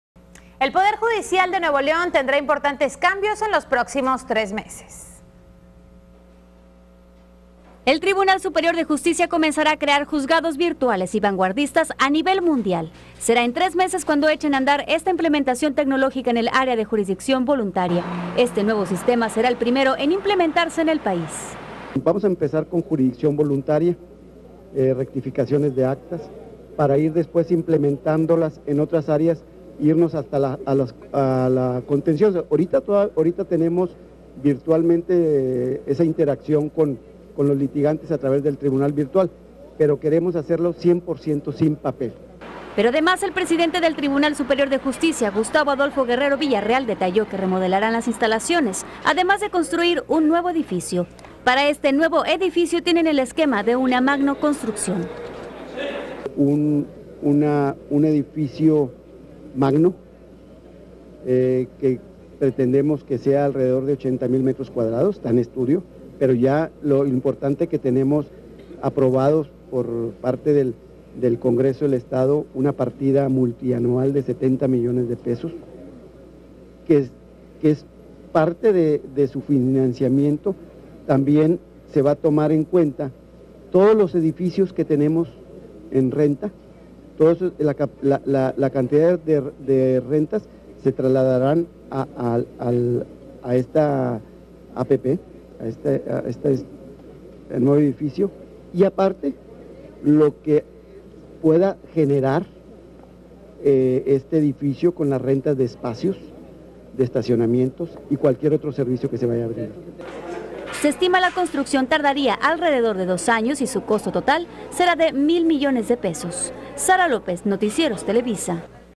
Las Noticias - Modernizarán Poder Judicial
El Tribunal Superior de Justicia comenzará a crear Juzgados virtuales y vanguardistas a nivel mundial.